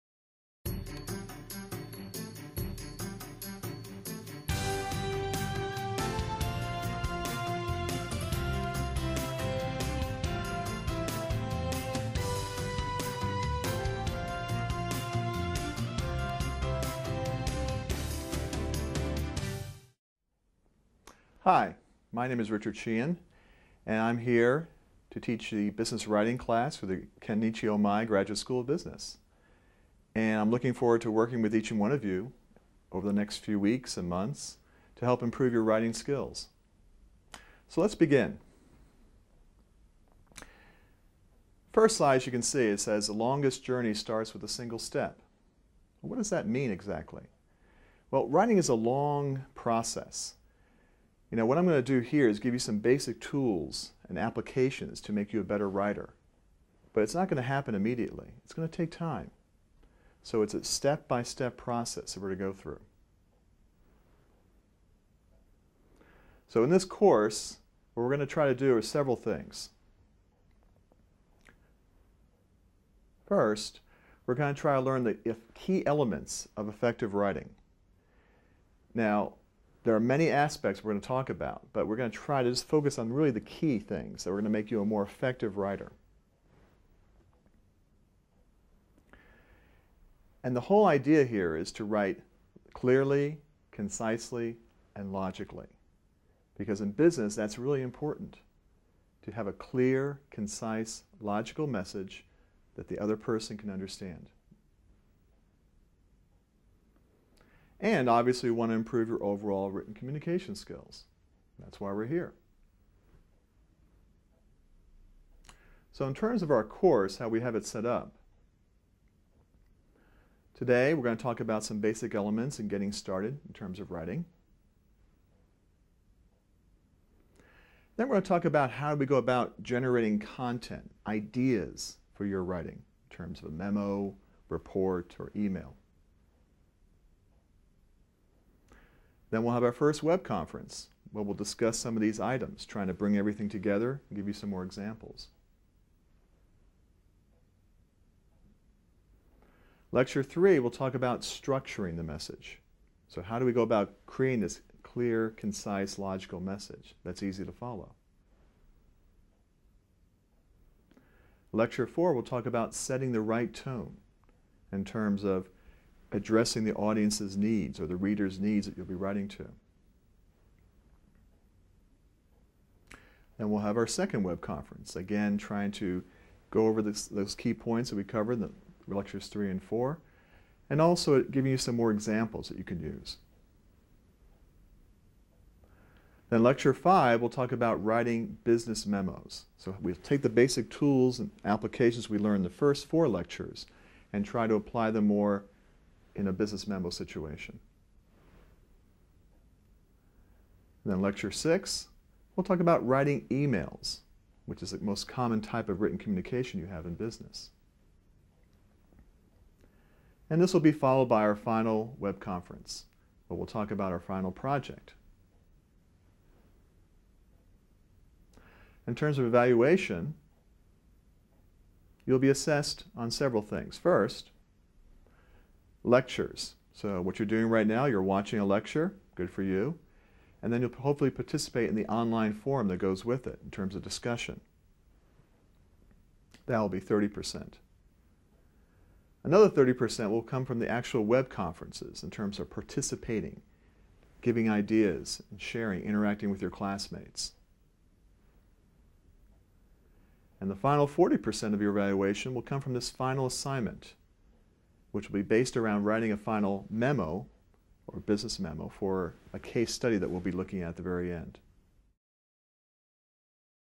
Kenichi Ohmae Graduate MBA in Globalization Business Writing
This lecture is part of a series on business writing skills, taught at the Kenichi Ohmae Graduate School of Business.
ビジネスブレークスルー大学院大学
ＭＢＡグローバライゼーション専攻